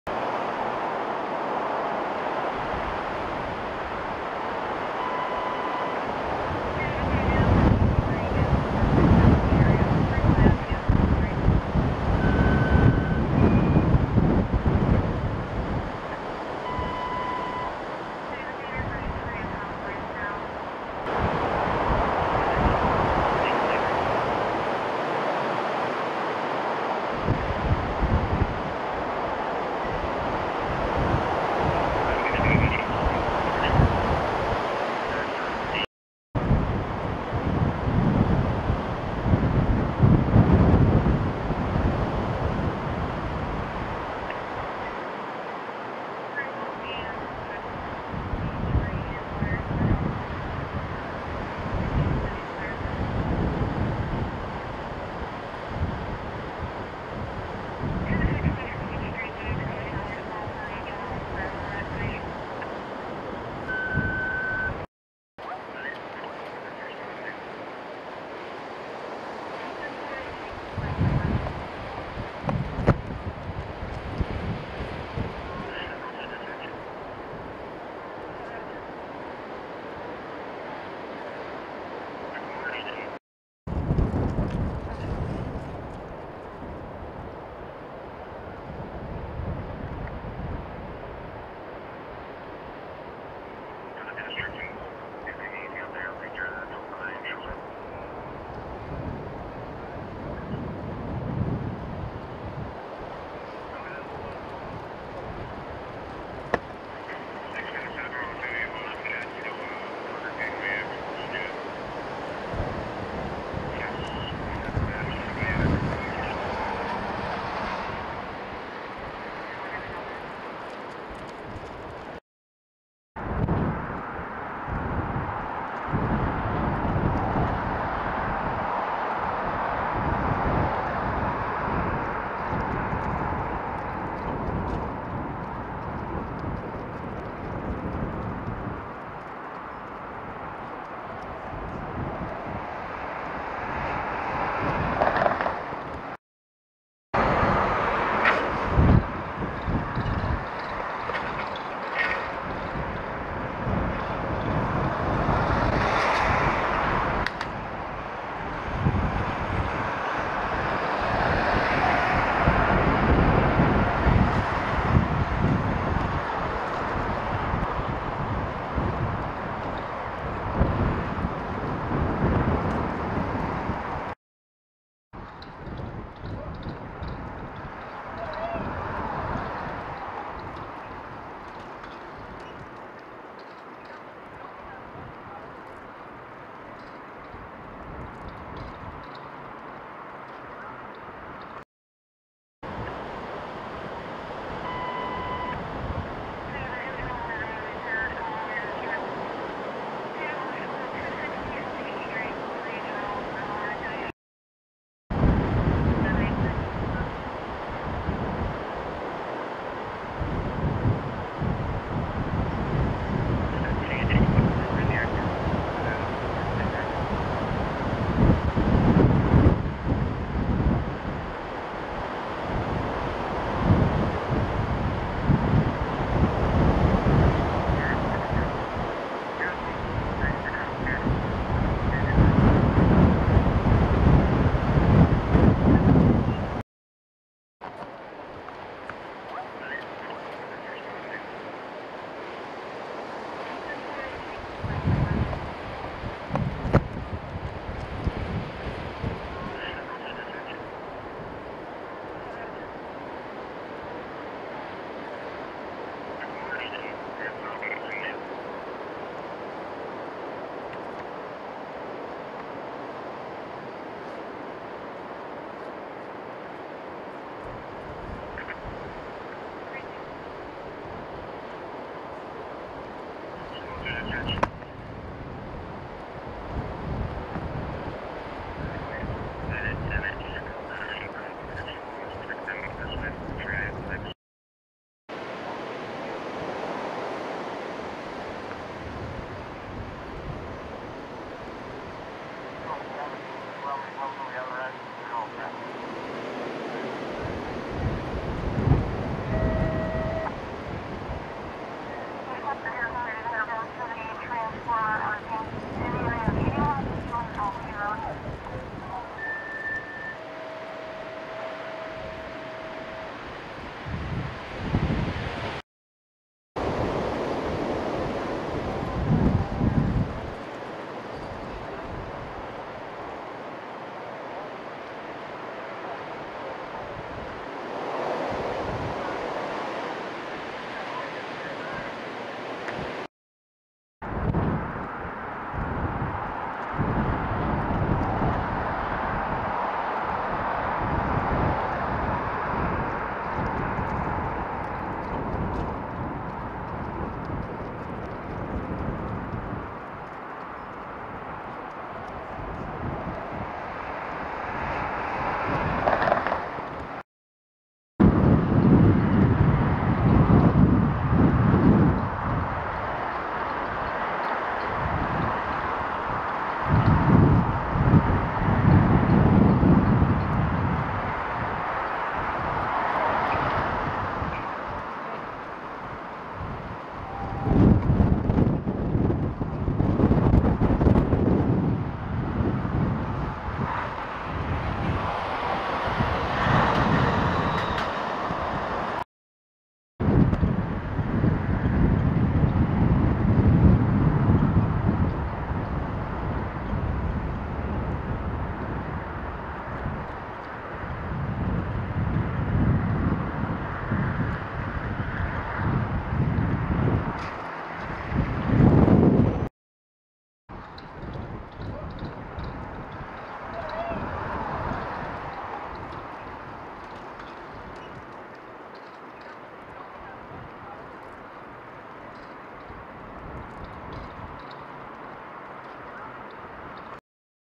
BATAVIA/High winds cause damage and downed power lines, trees and outages keep emergency crews busy
Wind gusts of up to 60 mph were recorded in the Batavia area causing damage and power lines, poles and trees to come down.

According to National Grid, 2,479 customers are without power in the Batavia area. The estimated restore time is 11:45p.m.